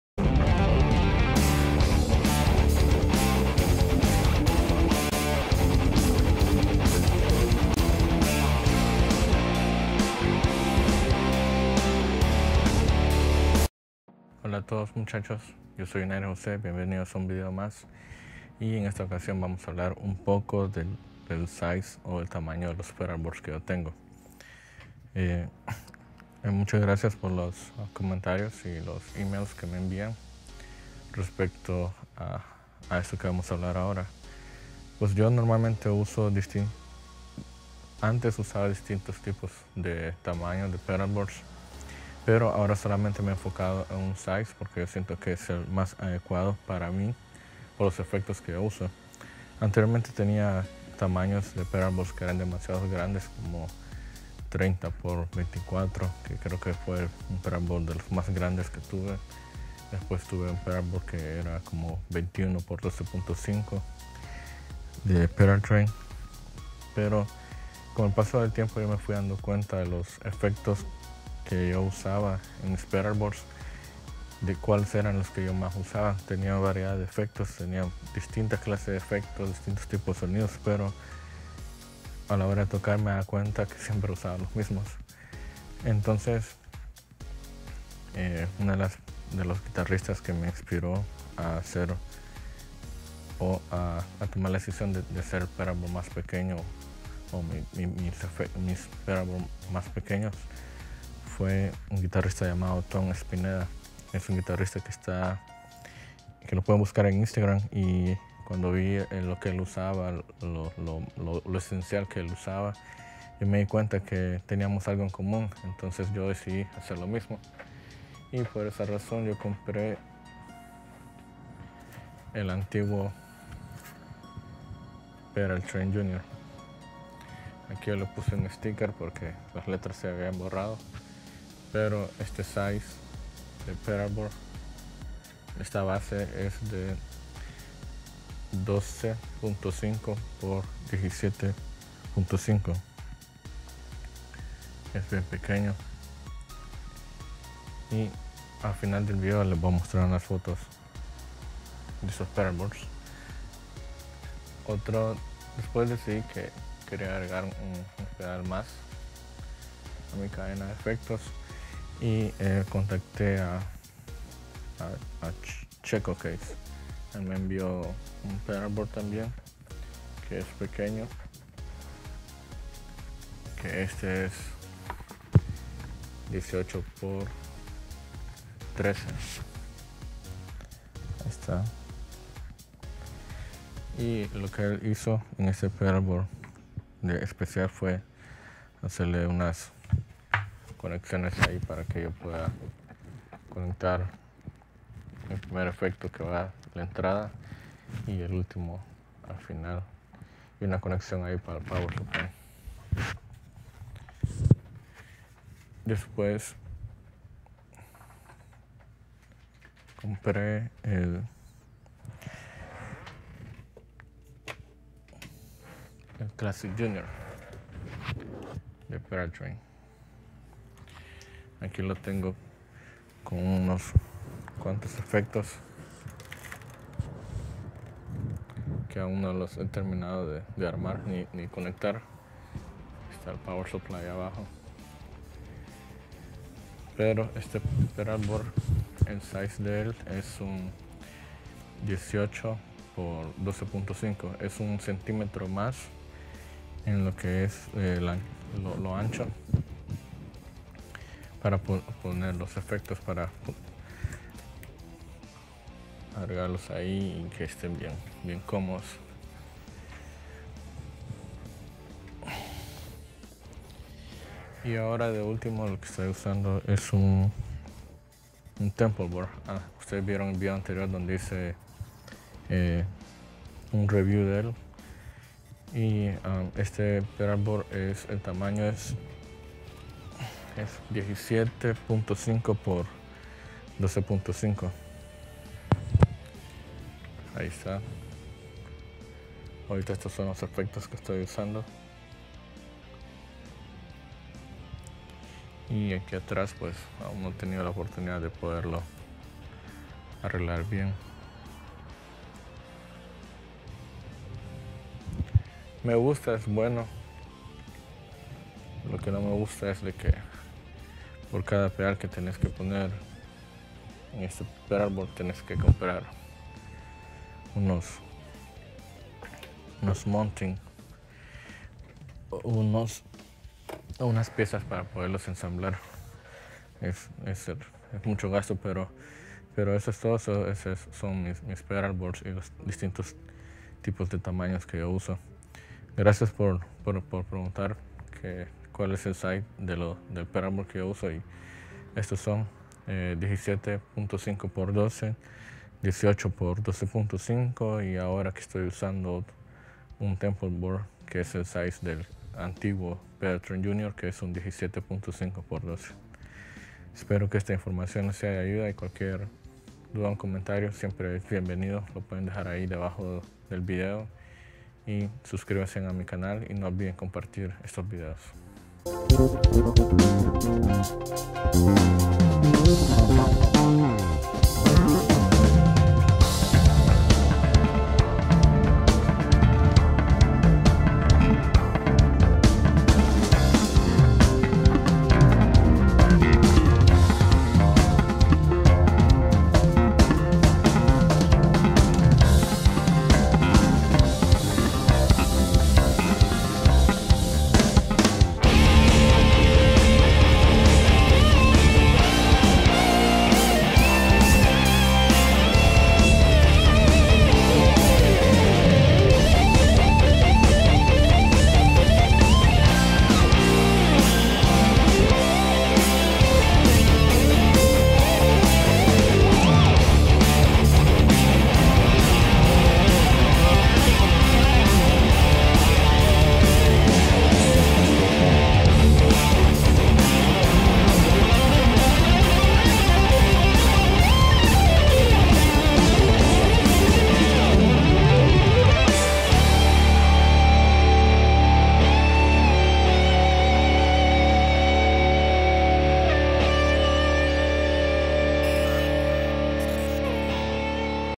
SIZE DE PEDALBOARDS
Muchos de ustedes me han preguntado antes Cual es el size(tamaño) de la base que uso para mis efectos. Bueno les dejo este pequeño video espero les sea de ayuda.